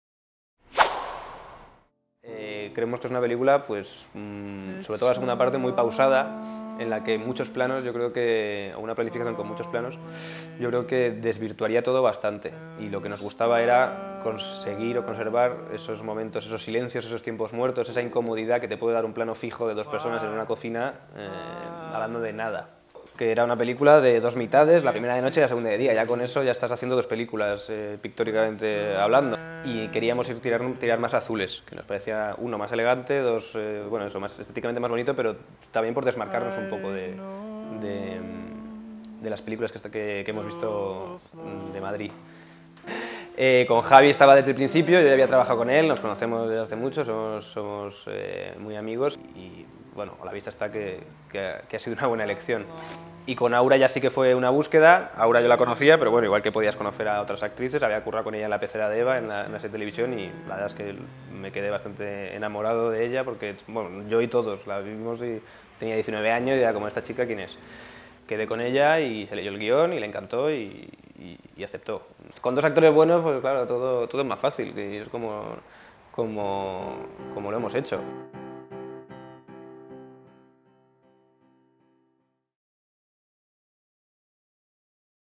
Entrevista a Rodrigo Sorogoyen (Director de Stockholm)
Grabación, Montaje y Edición de una serie de entrevistas efectuadas por Alfonso Caro para la Web de Cine realizadas a su Director y Co-Guionista RODRIGO SOROGOYEN, al Co-Protagonista (junto a AURA GARRIDO) JAVIER PEREIRA, y a la Co-Guionista ISABEL PEÑA, con motivo del estreno de la película STOCKHOLM.